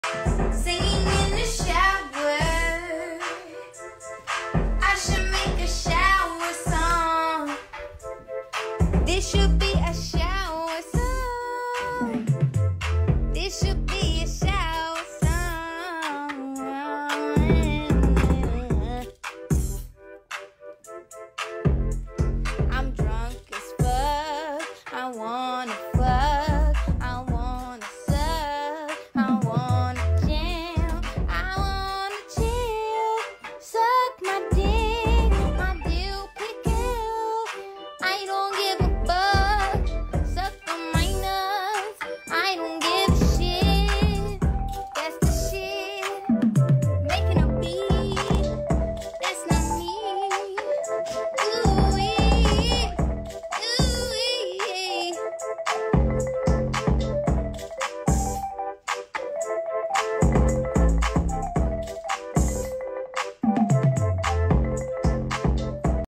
Doja cat shower song
Drunk Doja singing on live IG